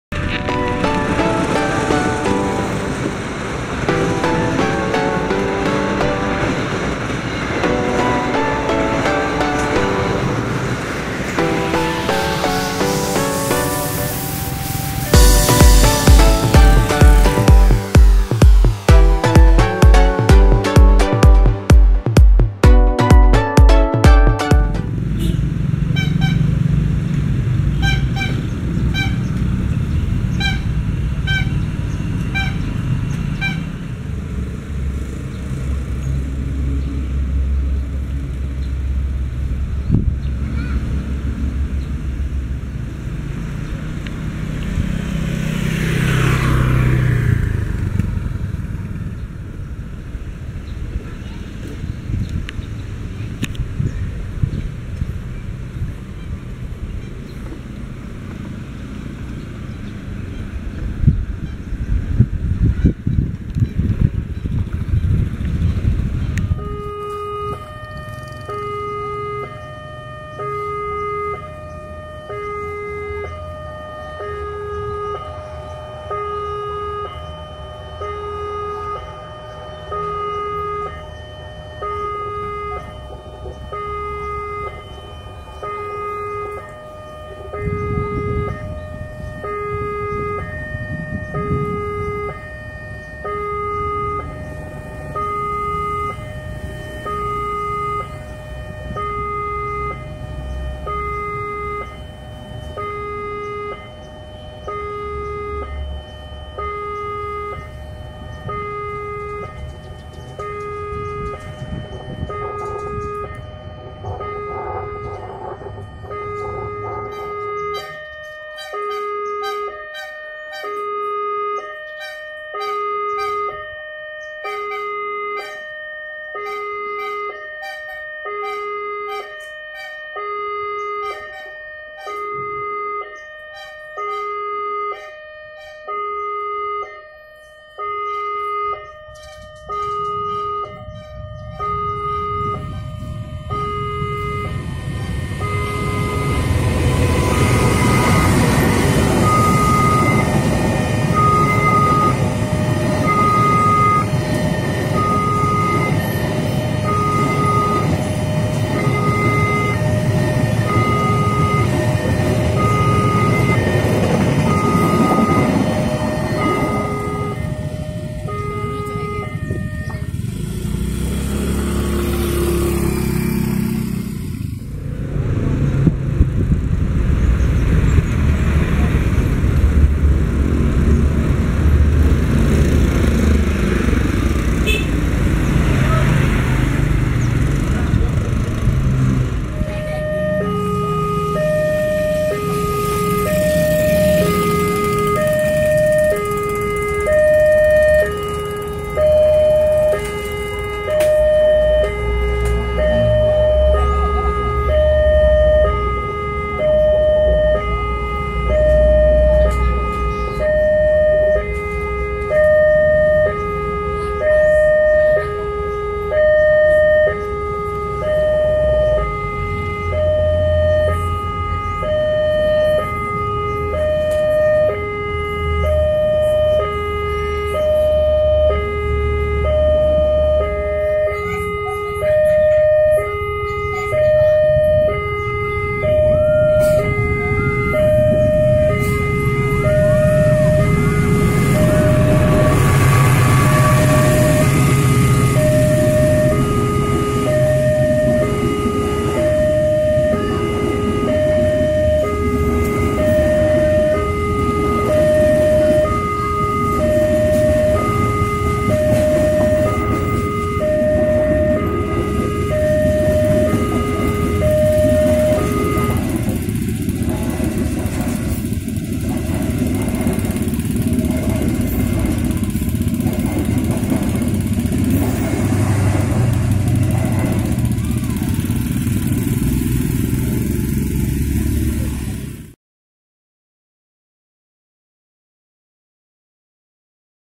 PALANG PARKIR Perlintasan Kereta Api Unik Gg 1 Tirto Pekalongan
perlintasan kereta api unik ini berada di gg 1 tirto pekalongan persis di area sinyal masuk stasiun pekalongan.

Model palang yang digunakan cukup unik  yaitu menggunakan palang pintu masuk parkir yang biasanya dipasang di mall atau pusat perbelanjaan.

Sayangnya lampu kedip sudah lama rusak dan hanya 1 sisi palang saja yang masih nyala itupun cuma 1 lampu

JPL ini dijaga oleh dinas perhubungan kota pekalongan

Kereta api yang melintas :
1. Ka Argo Bromo Anggrek
2. Ka Tawang Jaya